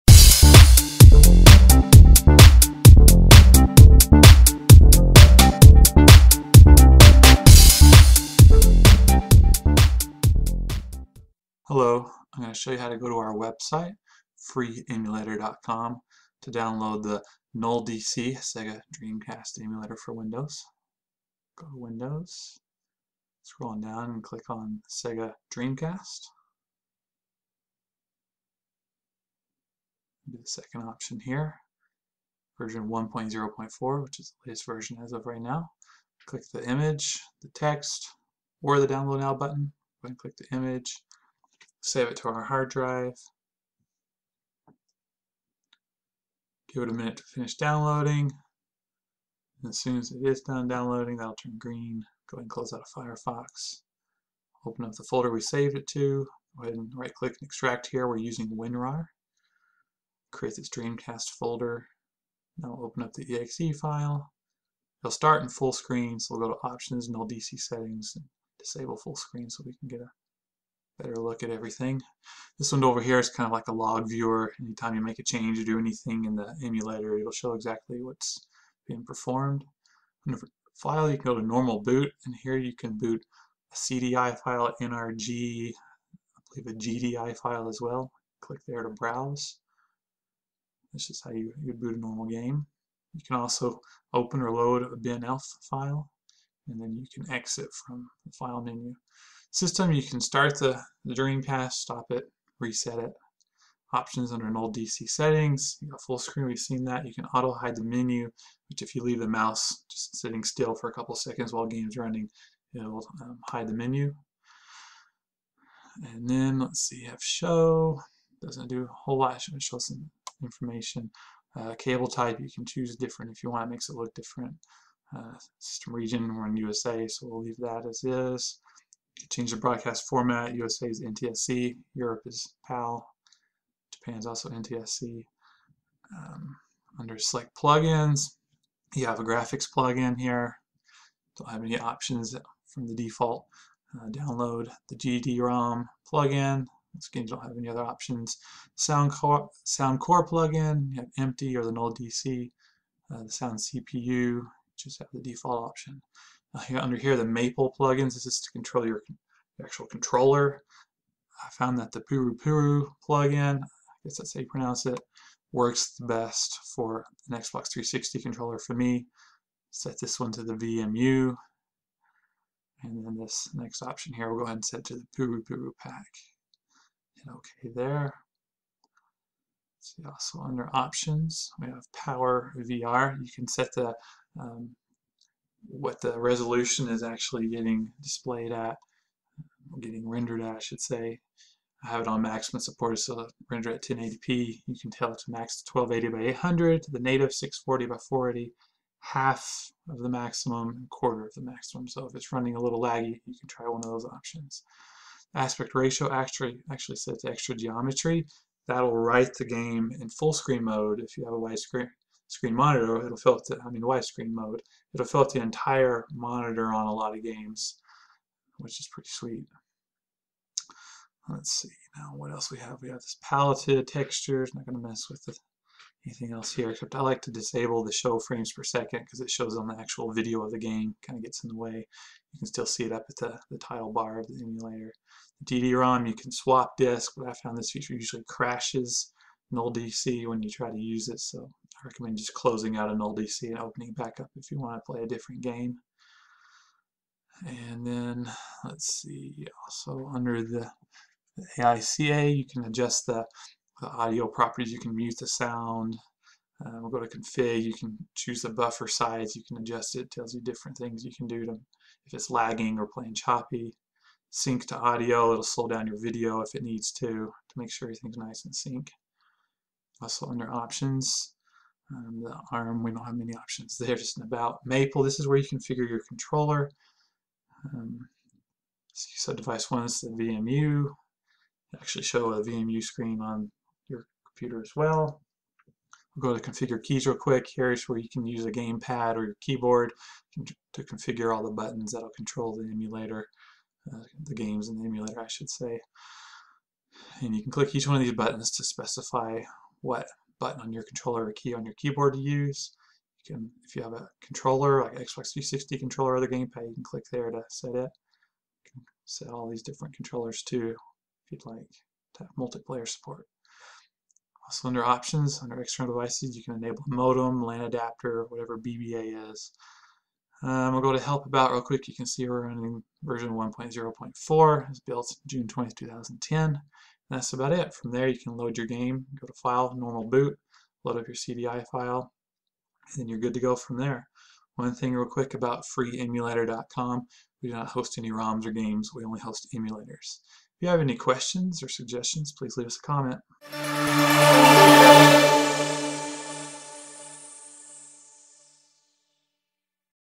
Download & Settings nullDC Emulator 1.0.4 for Sega Dreamcast on Windows [1080p HD]
Download Sega Dreamcast nullDC 1.0.4 Emulator

In this installation tutorial video we show you how to download and quickly configure the Sega Dreamcast emulator called nullDC for Windows (version 1.0.4).

With nullDC you can play all your favorite games, such as SoulCalibur, NFL 2K1, Resident Evil: Code Veronica, Rayman 2: The Great Escape, Quake III Arena, Jet Grind Radio, Sonic Adventure 1 & 2, Virtua Tennis, Marvel vs. Capcom 2, Shenmu, Ready 2 Rumble, and tons more!

If you need WinRAR to extract any files from the video, you can get it from here: